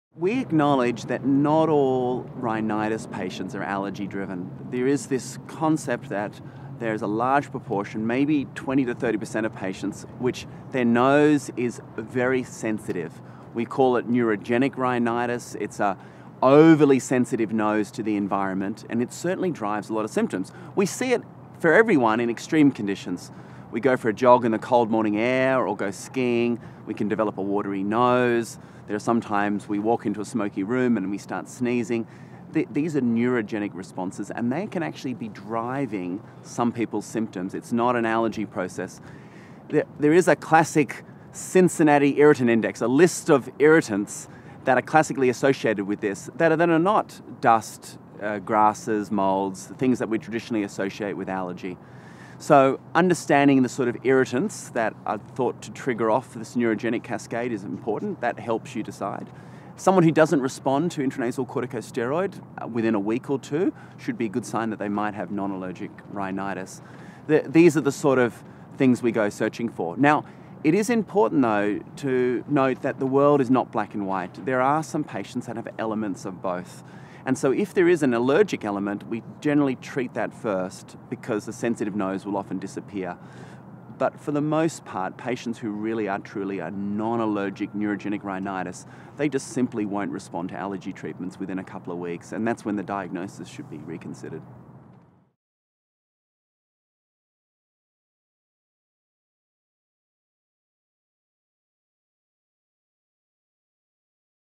Non allergic or neurogenic rhinitis
Prof Richard Harvey, Sydney NSW, speaks from the inaugural Mastering Allergy & Asthma GP Meeting held in Sydney late 2017, that featured international and Australian presenters. Independent, interactive and activity-focused, the meeting featured presentations, expert panel and group discussions, and workshops.

To stay in touch with our research and activities on nose and sinus disorders based in Sydney: